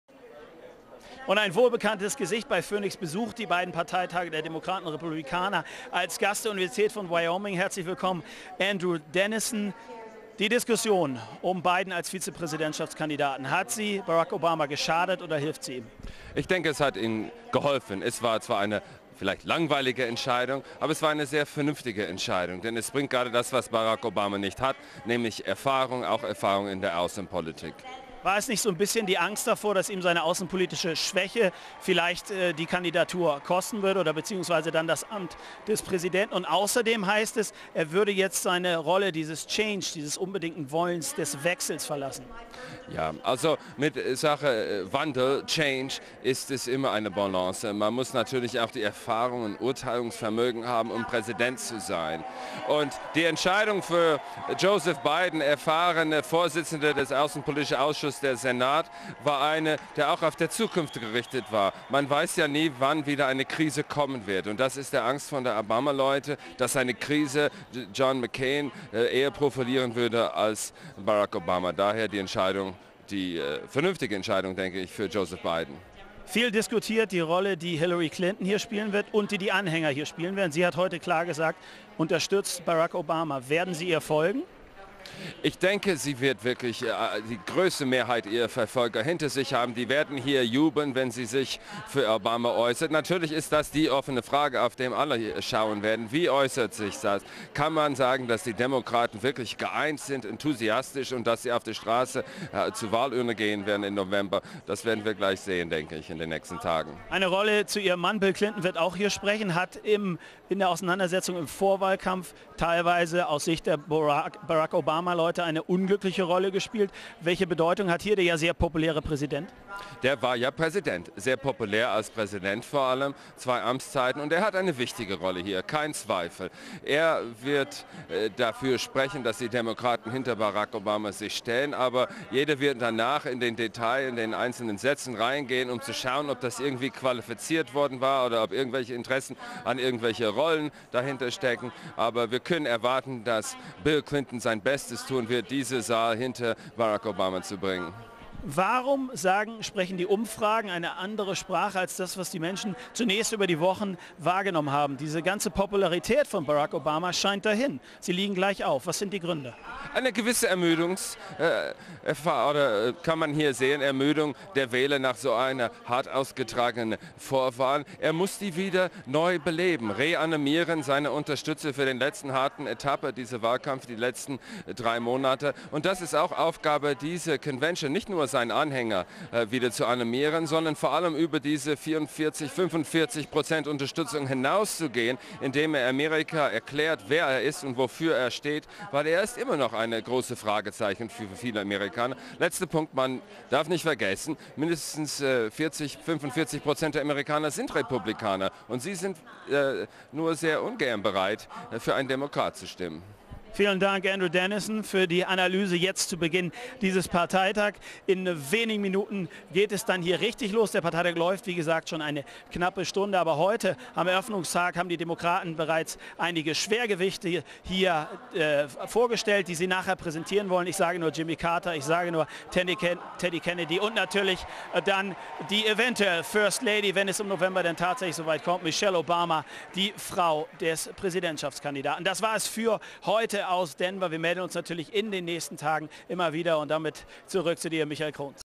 Andrew Denison / PHOENIX: Die Democratic Convention beginnt
Im Rahmen der ausführlichen Berichterstattung von PHOENIX kommentiert Dr. Andrew B. Denison immer wieder live aus Denver.